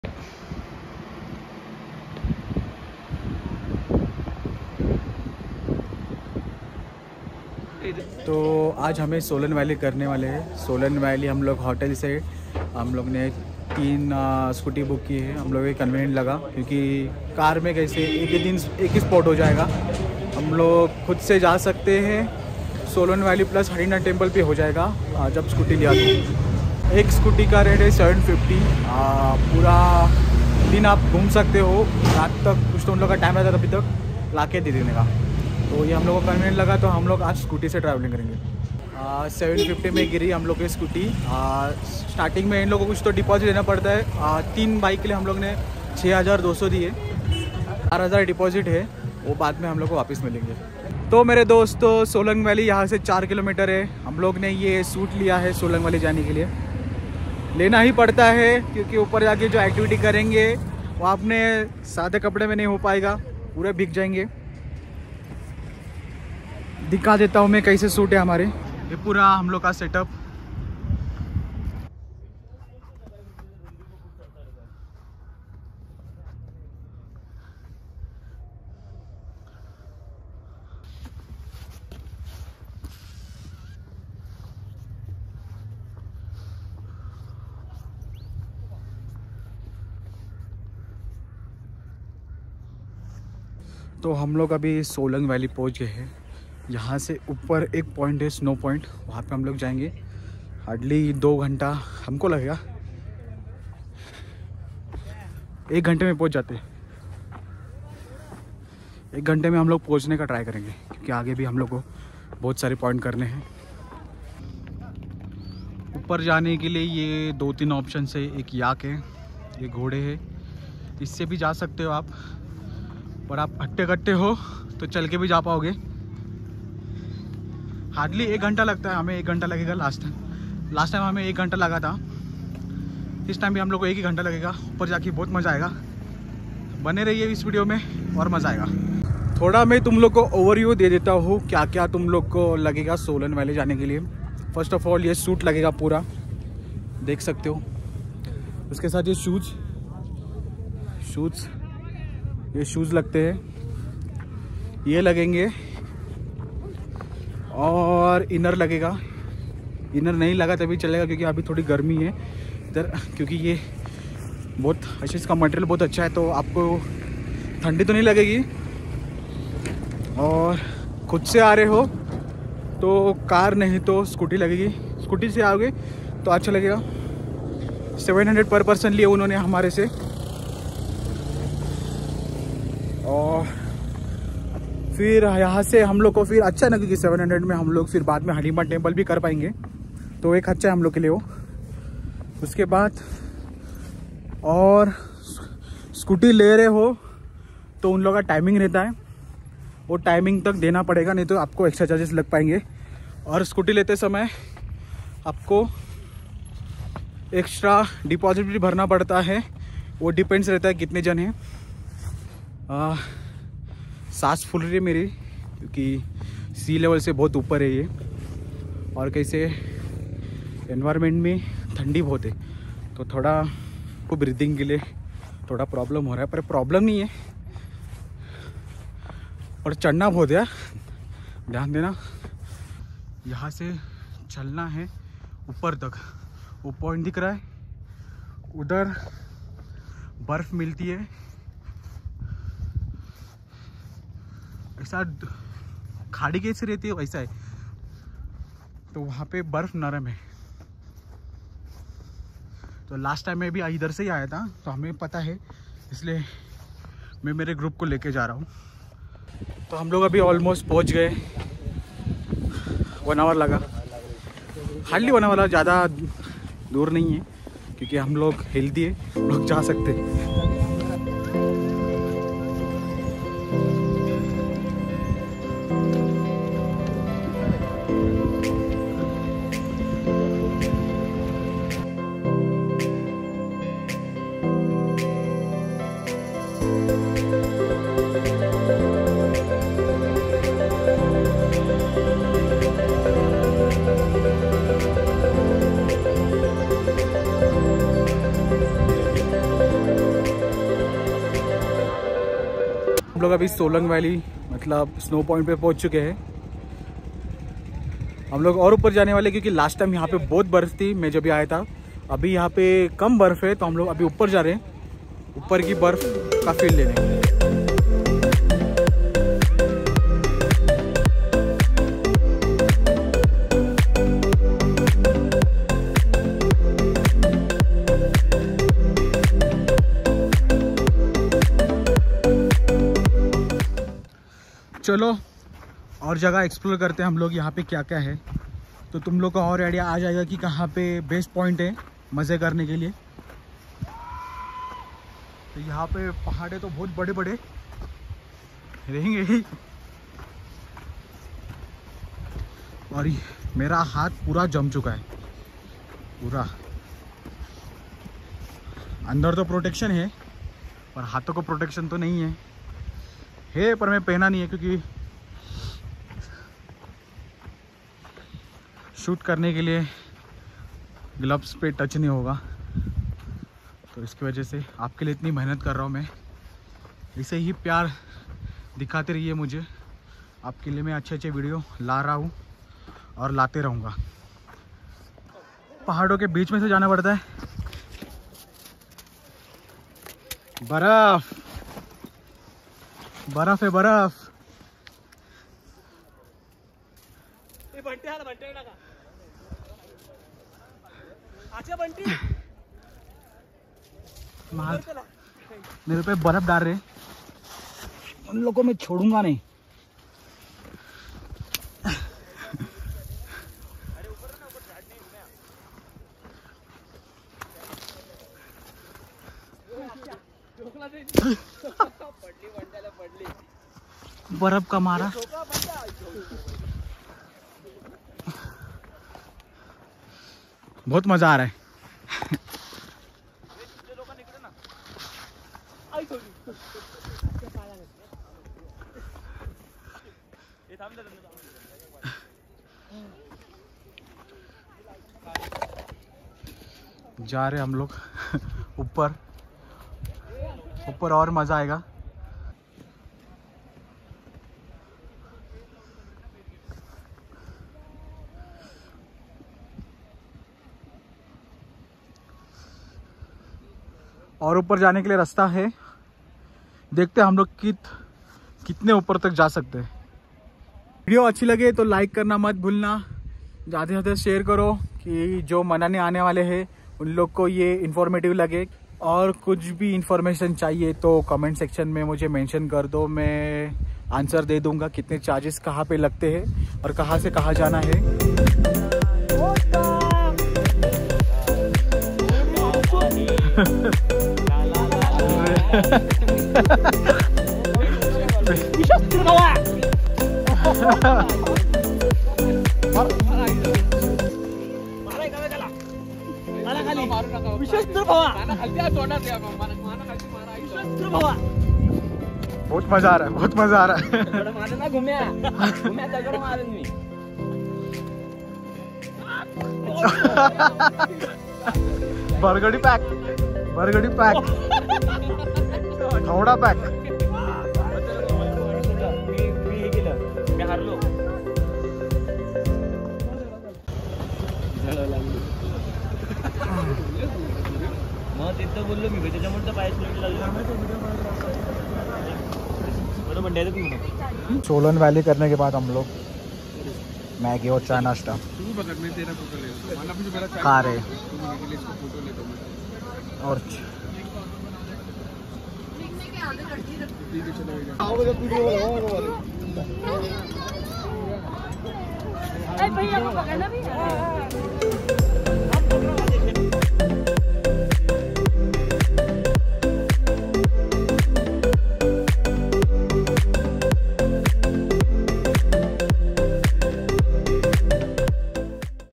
Solang Valley - Manali | Snow Valley
Solang Valley derives its name from combination of words Solang and Nallah. It is a side valley at the top of the Kullu Valley in Himachal Pradesh